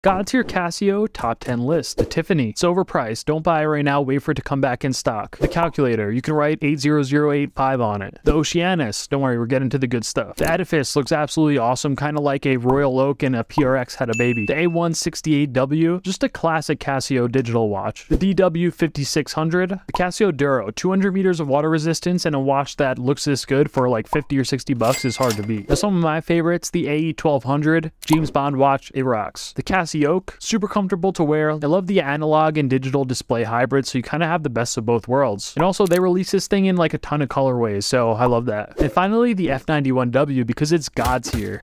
10 God Tier Casios
10 god-tier casio watches that deserve their hype. Shoutout Nico.

Casio Calculator
Casio Oceanus
Casio Edifice
Casio A168W
Casio DW5600
Casio MDV106 "Duro"
Casio AE1200
Casio F91W